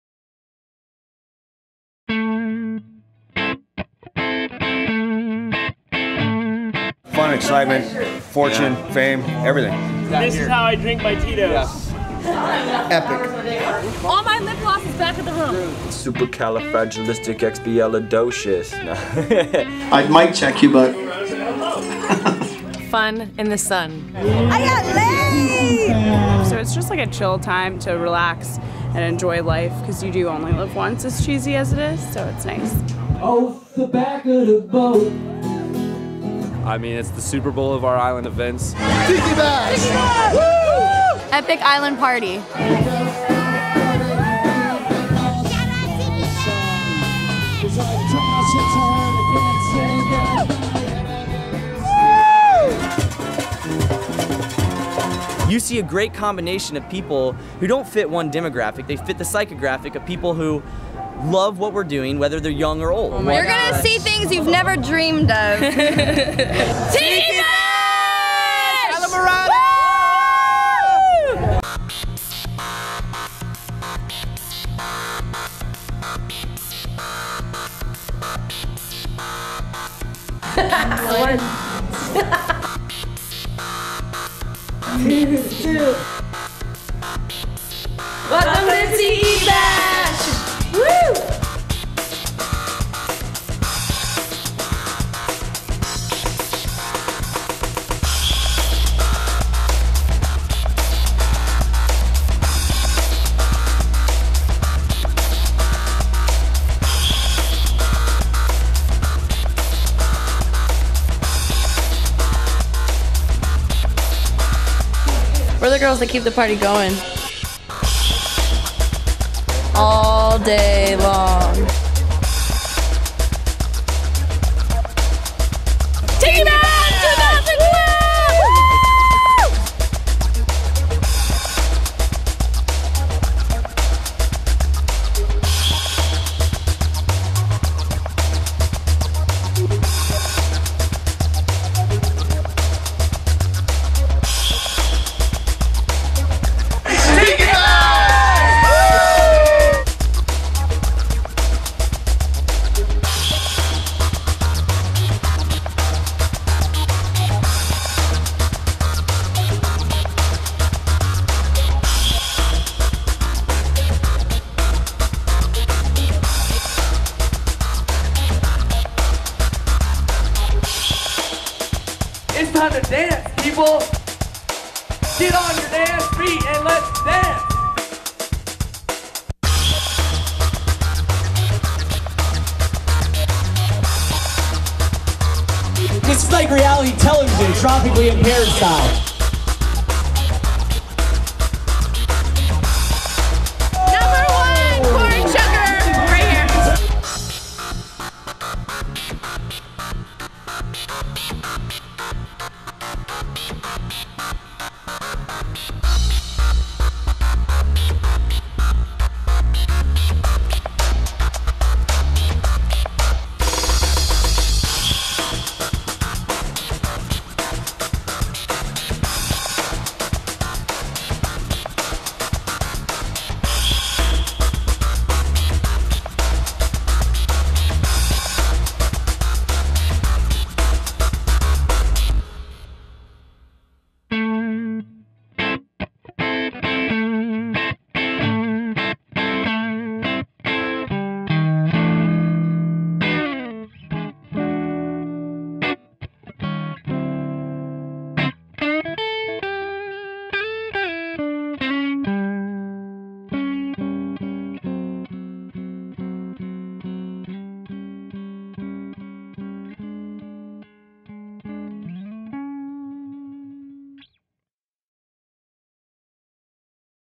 Tiki Bash Islamorada 2012!
Check out some of the craziness and fun after an epic year at the 8th Annual Tiki Bash Islamorada!  For more information on how you can join the party go